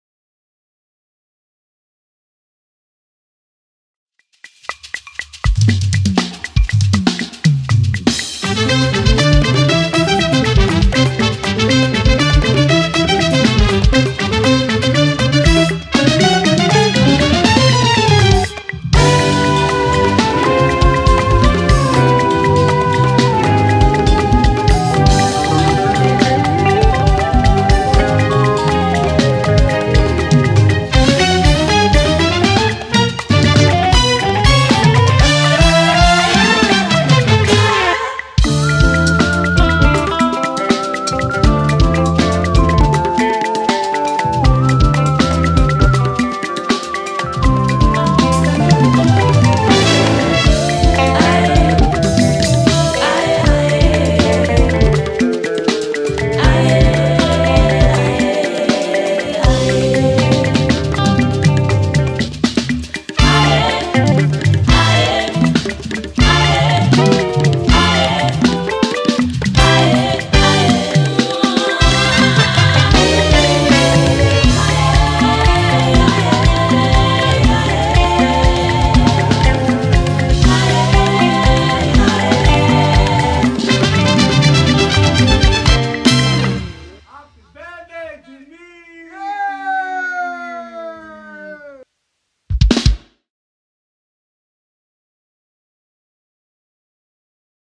FRANCIS Kwesi Mensah's BIRTHDAY JAM
Happy birthday jam with my friends
@nee_dromor on keys
@MyBlankson  on bass
@therealtuga8990 on guitars
@Nanaseibu on congas
@richmondbilson9064 on horns